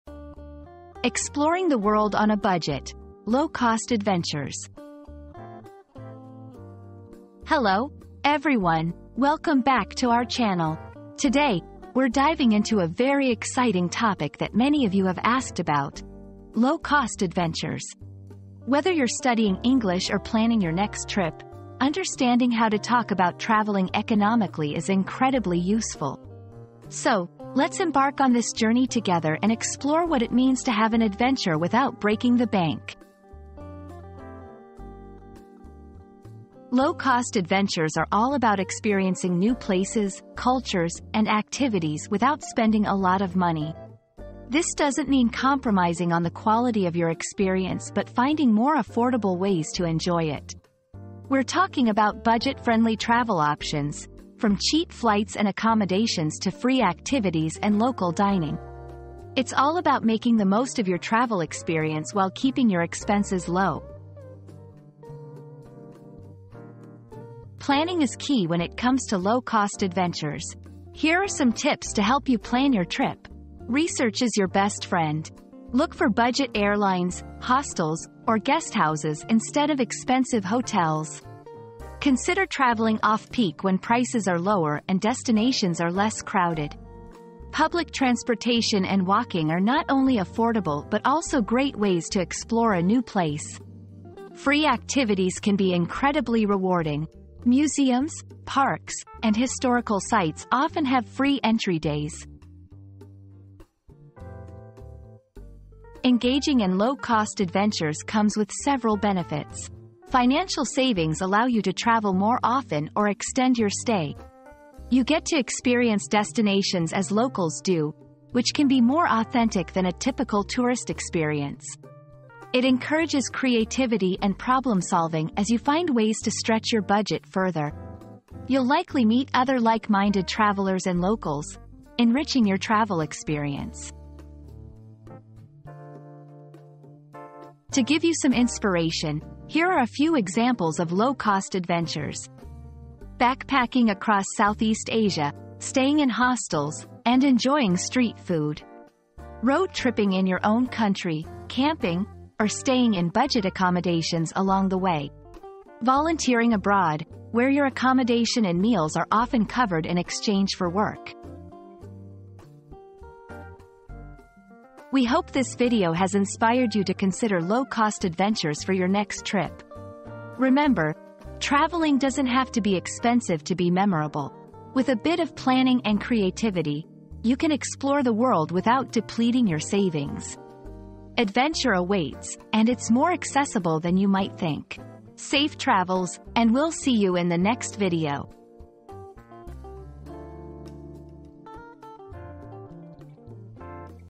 Exploring the World on a Budget: Low-Cost Adventures
Budget-Friendly Adventures: Explore the World Without Breaking the Bank • Join us as we embark on exciting low-cost adventures around the world, proving that you don't need to spend a fortune to have an unforgettable experience.

00:00 • Introduction - Exploring the World on a Budget: Low-Cost Adventures
00:36 • Understanding "Low-Cost Adventures"
01:07 • How to Plan a Low-Cost Adventure
01:45 • Benefits of Low-Cost Adventures
02:17 • Examples of Low-Cost Adventures